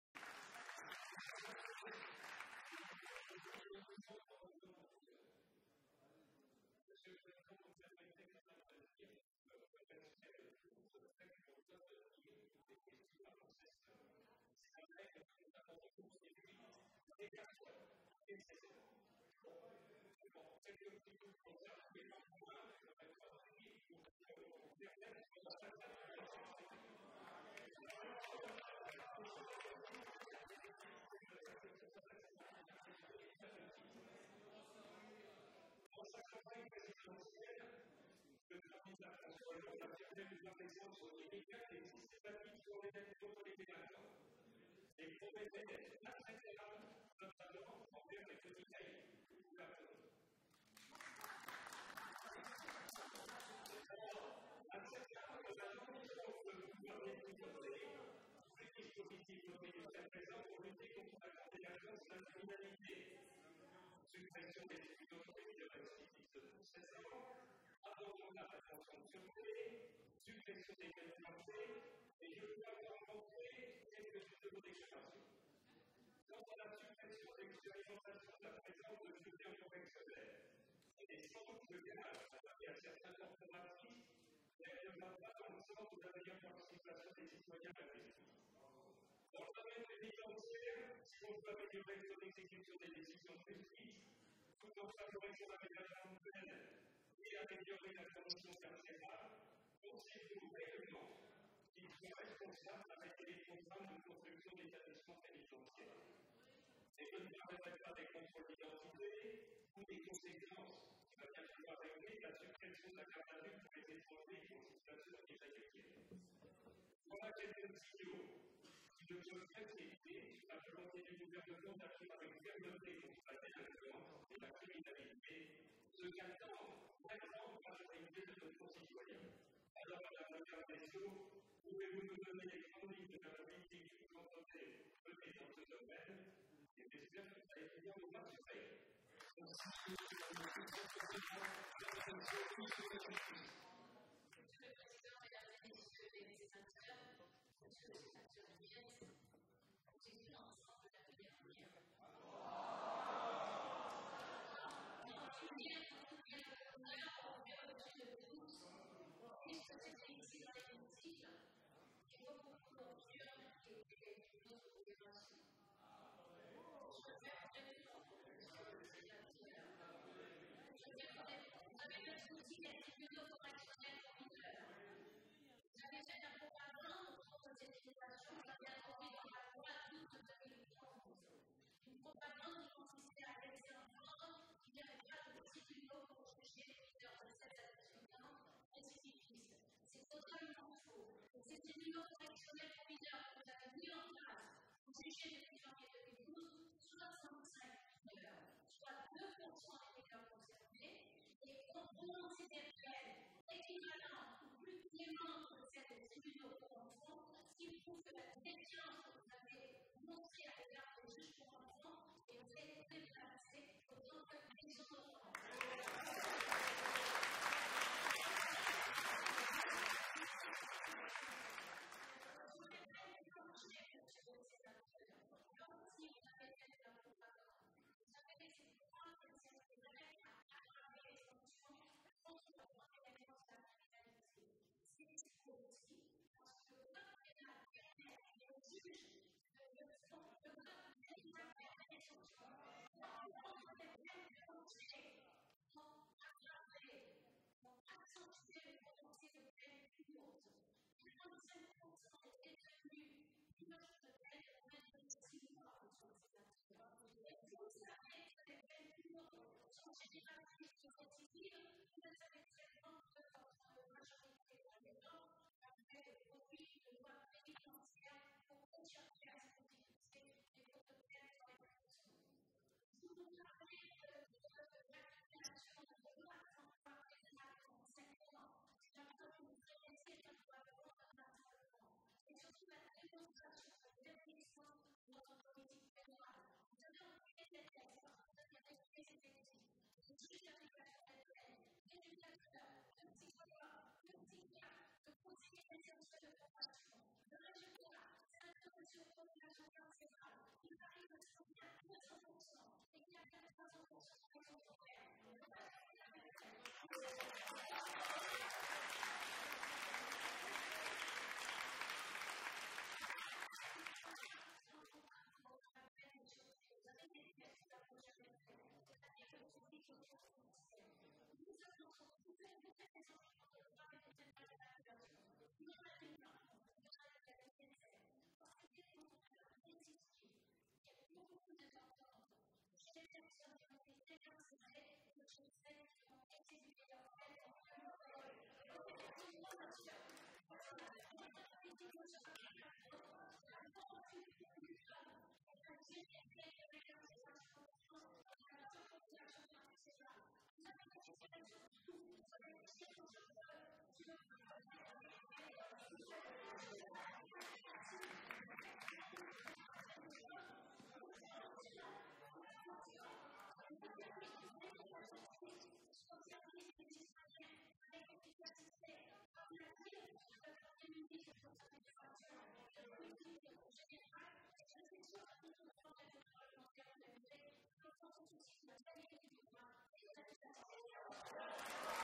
Jean-Jacques Hyest : Politique du ministère de la justice
Jean-Jacques Hyest (Seine-et-Marne) interroge la ministre de la Justice, Christiane Taubira, lors des questions d'actualité au gouvernement au Sénat.